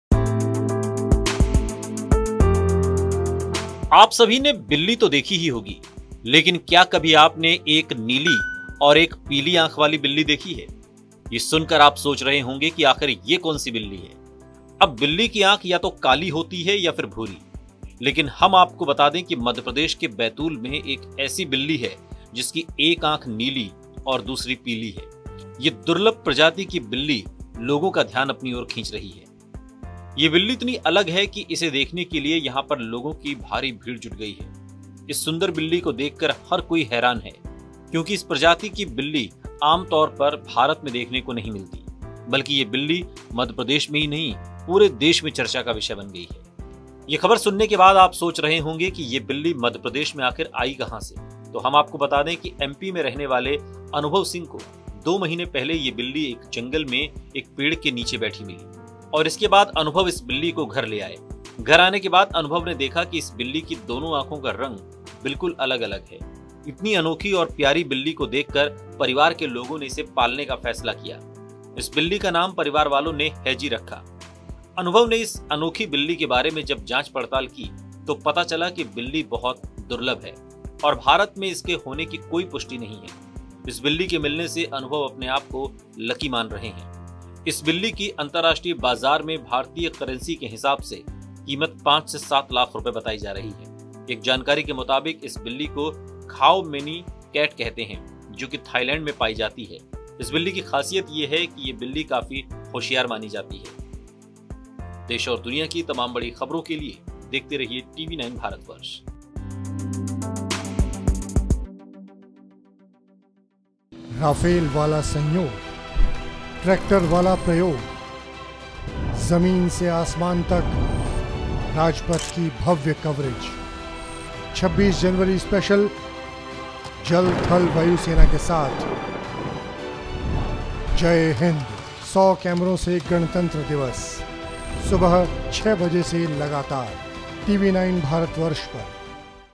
Madhya Pradesh के जंगल में मिली दुर्लभ प्रजाति की Cat, एक आंख नीली और दूसरी पीली...
मध्य प्रदेश (Madhya Pradesh) के बैतूल (Betul) में एक दुर्लभ प्रजाति की बिल्ली (Cat) लोगों की दिलचस्पी की वजह बनी हुई है. ये बिल्ली इतनी अलग है कि इसे देखने के लिए यहां लोगों की भारी भीड़ जुट रही है. सफेद रंग की इस बिल्ली (Cat) की एक आंख नीली है और दूसरी पीली. इस सुंदर बिल्ली को देखकर हर कोई हैरान हो रहा है, क्योंकि आमतौर पर इस प्रजाति की बिल्ली भारत (India) में देखने को नहीं मिलती. इसलिए ये बिल्ली (Cat) अब पूरे देश में चर्चा का विषय बनी हुई है.

Credit : National | Abhishek/Producer | Ekta/Editor